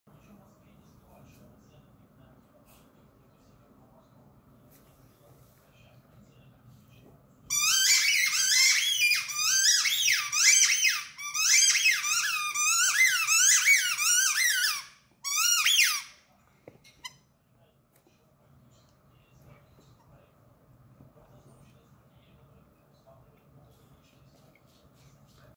Pay Attention to me🦊
Our Lazy Akita loves attention. He makes sure to get us to pay attention to him in different ways. He will find a way to get us to pay attention. He knows how much we adore loud squeaky sounds 😄🤣 He just wants love and food.